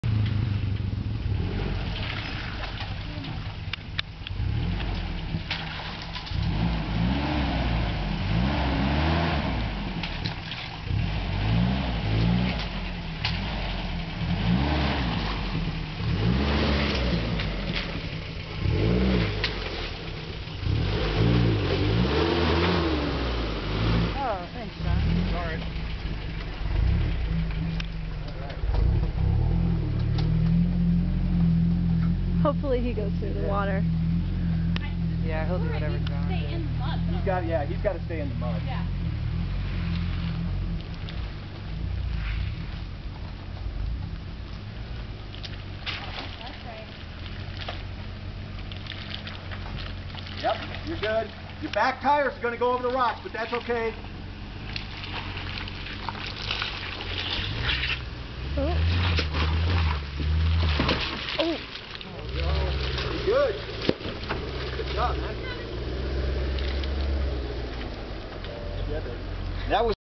4Runner and Touareg - Caribou Creek - Nederland, CO
Toyota 4Runner and VW Touareg running Caribou Creek trail from Ward to Nederland, July 2008.